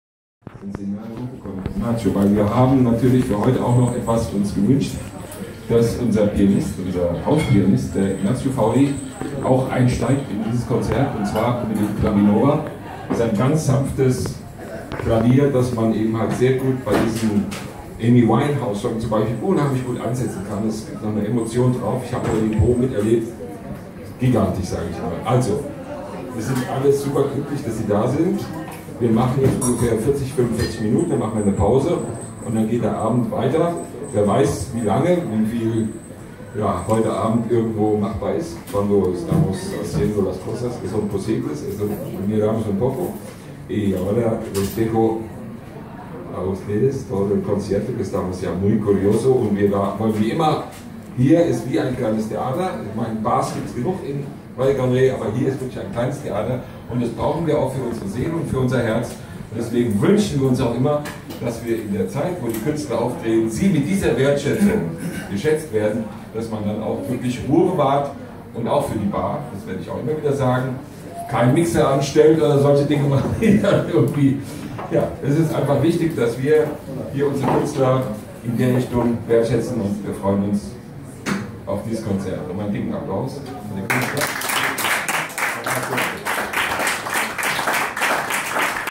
01 of 11 Introduction of Duo Regina Zerené y Omar Isai' - Live en GOMERA LOUNGE, Valle Gran Rey
Regina Zerené y Omar Isai' - Duo R&B Soul Jazz - Live en GOMERA LOUNGE, Valle Gran Rey, La Gomera, Canary Islands, Spain 5th of June 2017

Duo Regina Zerené (Palestine)  & Omar Isai (Mexico) with local Ignacio Fauri from Gomera at the piano

01 of 11: Introduction by Thomas Müller, Owner of GomeraLounge

No reference to the professional quality standards of SalamMedia!!